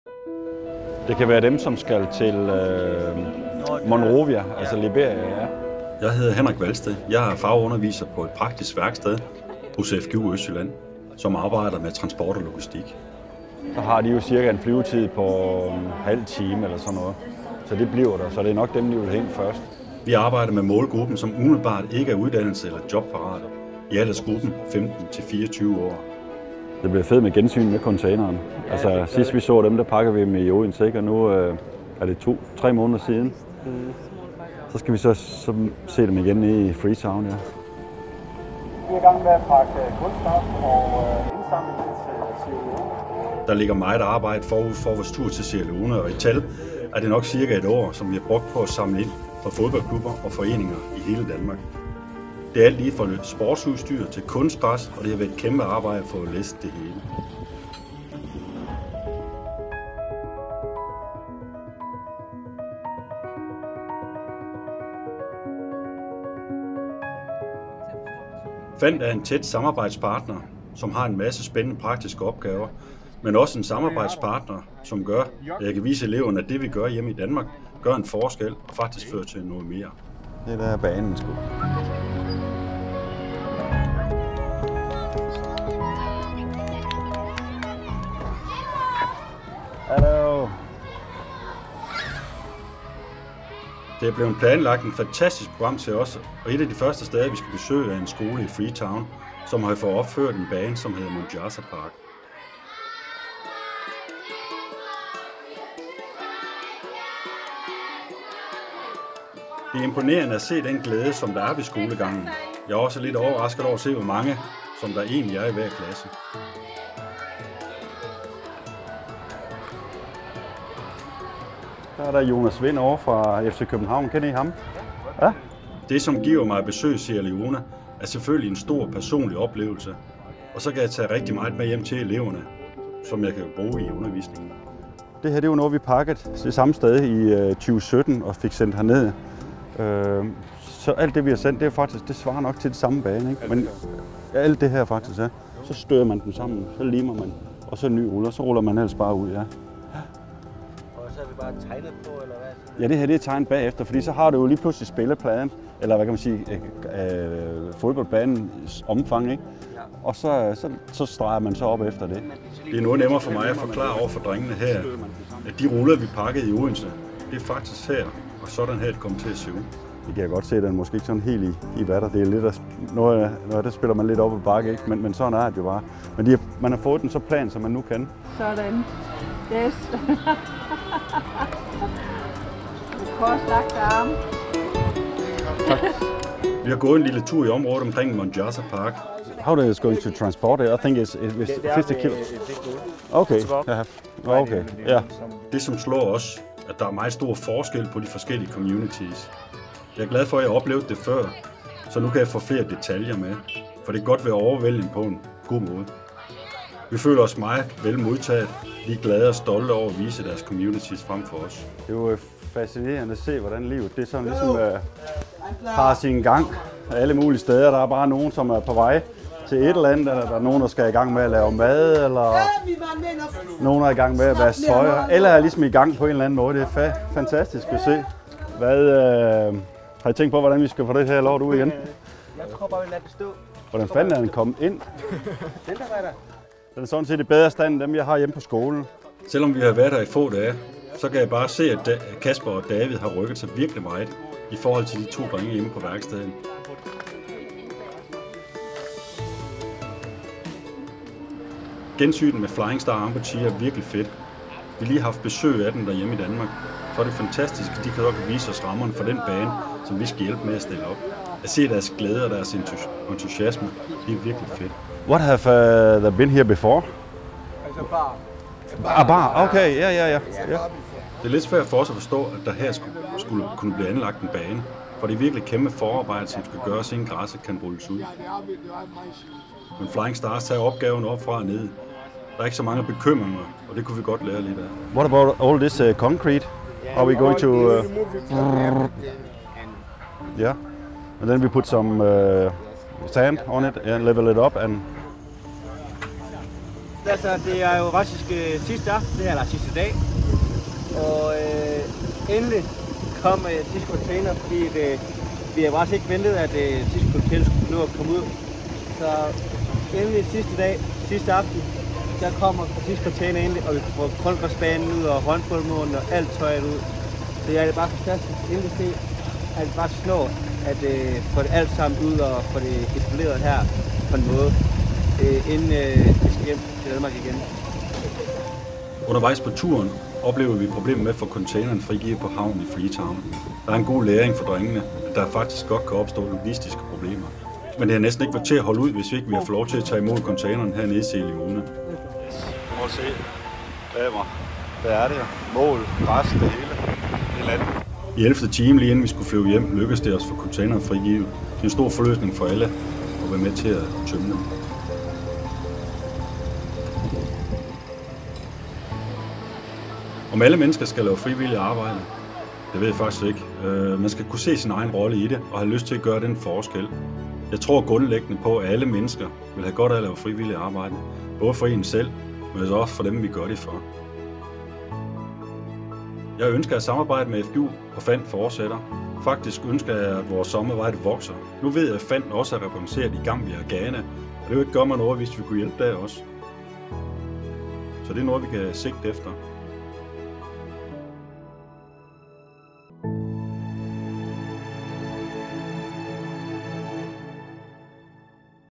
FANT x FGU Østjylland i Sierra Leone
Vi havde i oktober 2023 den store fornøjelse at byde vores mangeårige partner, FGU Østjylland, med på en tur til Sierra Leone som en del af vores fælles projekt under GLOBUS-puljen. Underviser på Transport og Logistik-linjen, Henrik Vahlsted, samt eleverne Casper og David brugte en uge i det vestafrikanske land på at modtage containeren med det indsamlede, brugte sportsudstyr fra Danmark samt besøge og deltage i aktiviteter med FANTs partnere. Kortfilmen giver et indblik i besøget og viser, hvordan et samarbejde mellem FANT og uddannelsesinstitutioner kan være givende for begge parter.

Produceret af: Thomas Vestergård Sørensen